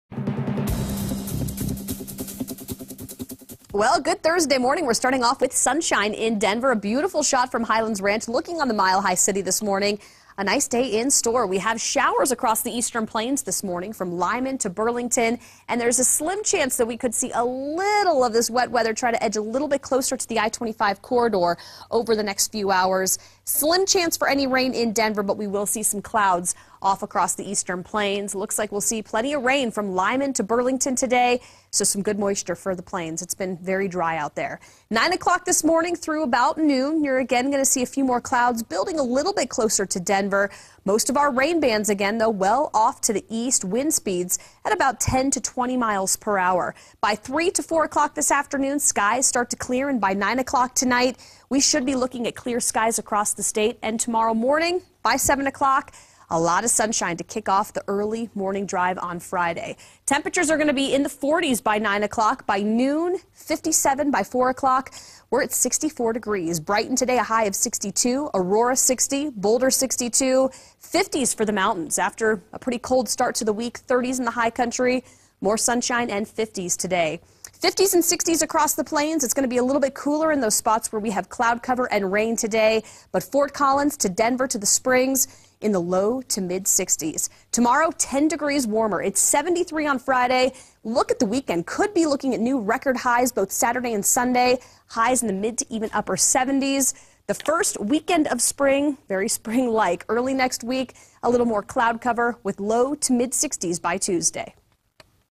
Mostly Sunny, Chance Of Light Rain
3/22/12 - Mostly sunny, with a chance of light rain east of I-25. Highs in the 60s.